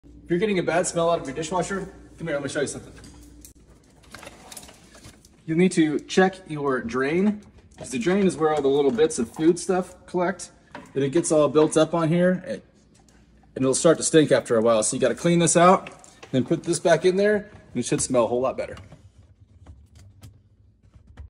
Bad smell from a dishwasher
if you're getting a bad smell out of your dishwasher, you'll need to check your drain because the drain is where all the little bits of food stuff collect that it gets all built up on here it and it'll start to stink after a while so you got to clean this out then put this back in there and it should smell a whole lot better.

This is a Kitchenaid dishwasher, but all dishwashers have some sort of drain that can get clogged with food particles.